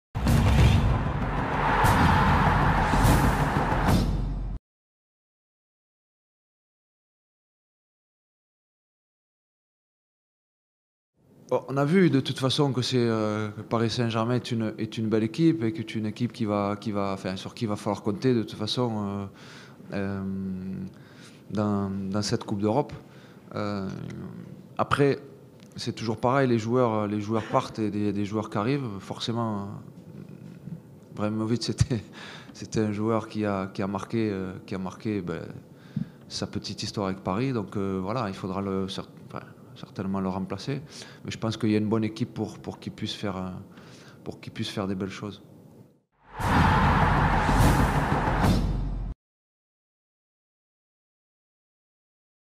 Zinedine Zidane juge le PSG sans Ibrahimovic
Opposé pour la première fois au Paris Saint-Germain depuis le départ de Zlatan Ibrahimovic, l'entraîneur du Real Madrid, Zinedine Zidane, a donné son point de vue sur les chances franciliennes de briller cette saison.